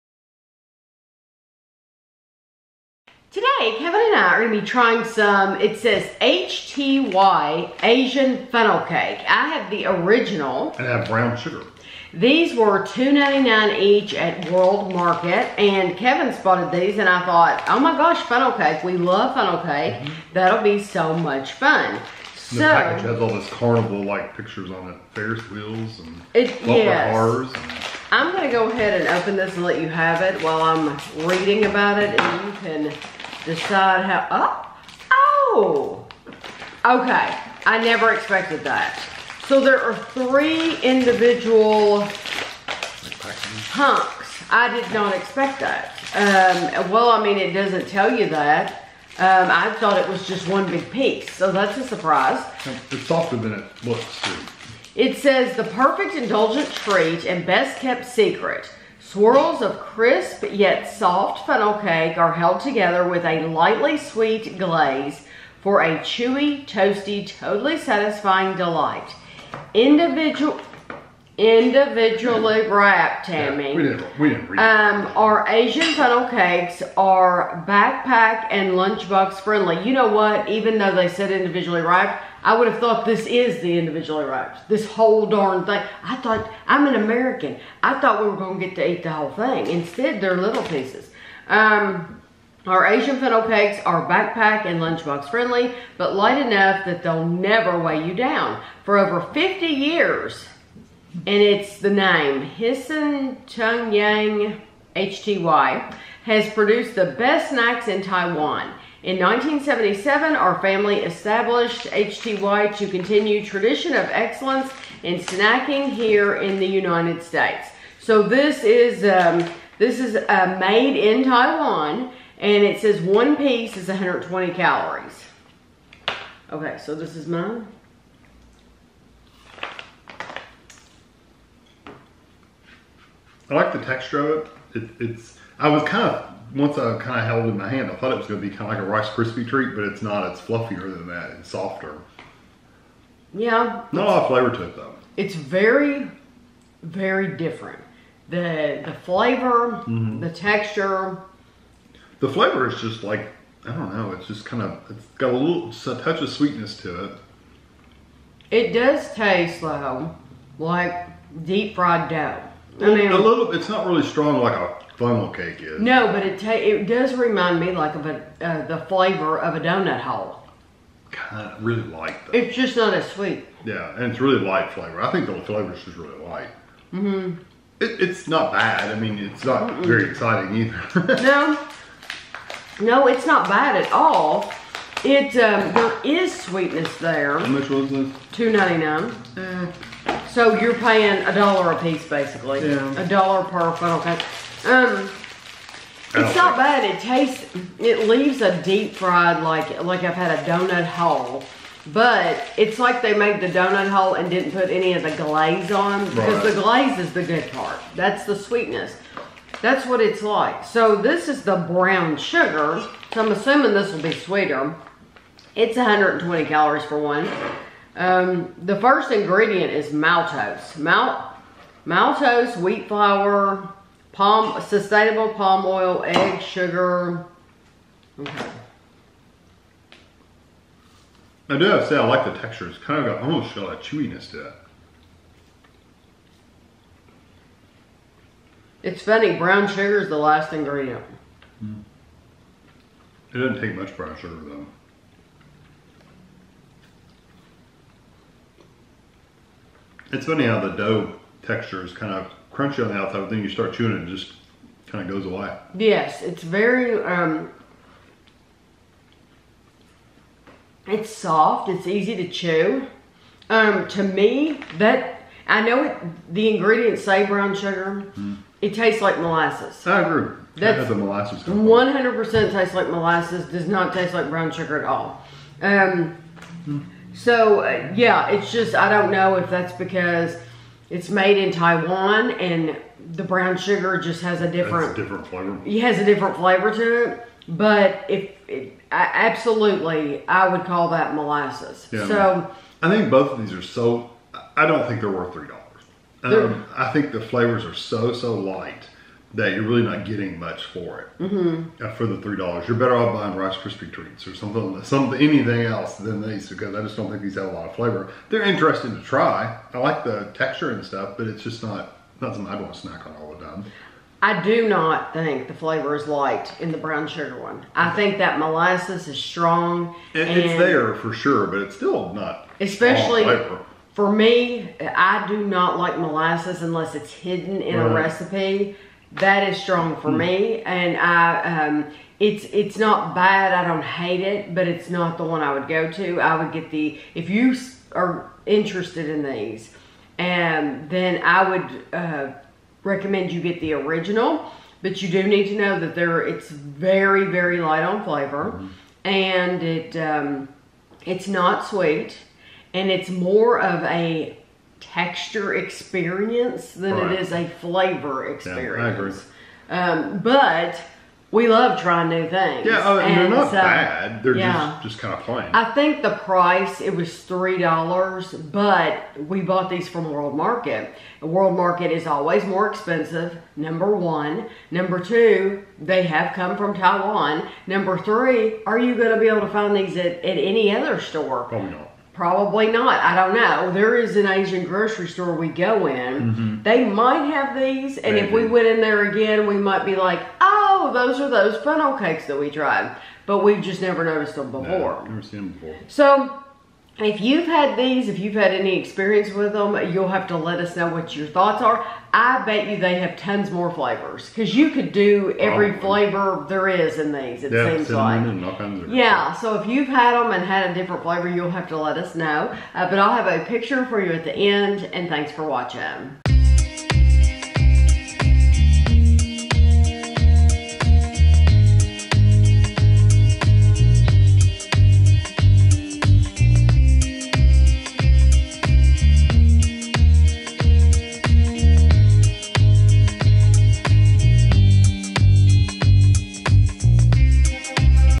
HTY Asian Funnel Cake: Original & Brown Sugar Review
This is a taste test/review of the HTY Asian Funnel Cake in the Original and Brown Sugar flavors. They were $2.99 each at World Market.